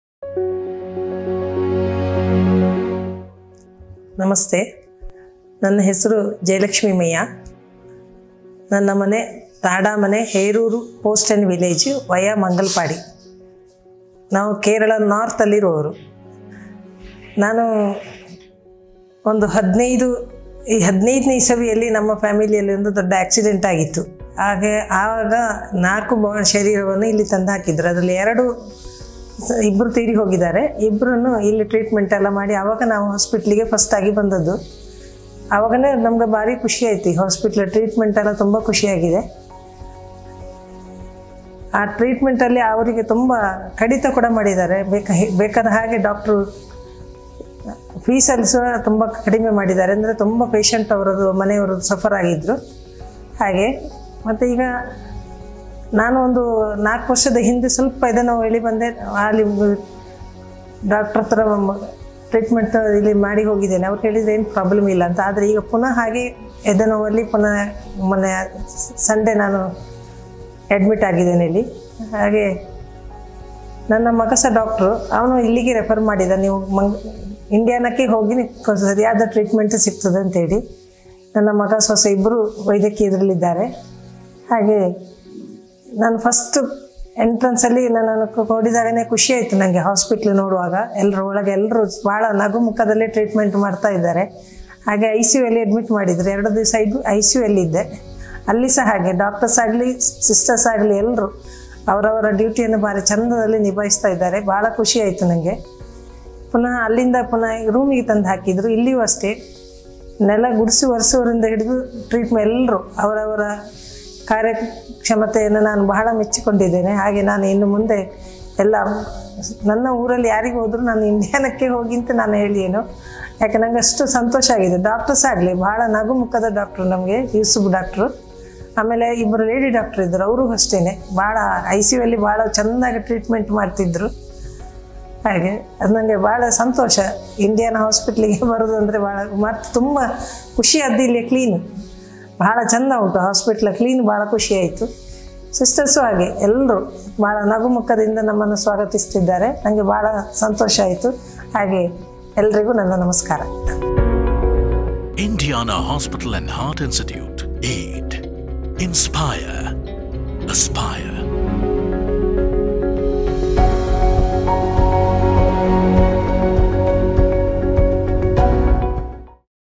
TESTIMONIAL | CARDIAC PATIENT | CARDIOLOGY | INDIANA HOSPITAL | HEART | DR YUSUF KUMBLE
A review from the patient at Indiana Hospital & Heart Institute, Mangaluru
They were happy with the treatment from our doctors, nurse and all staff of the hospital.

Dr. Yusuf Kumble, Cardiologist

to know more about Indiana Hospital and its facilities.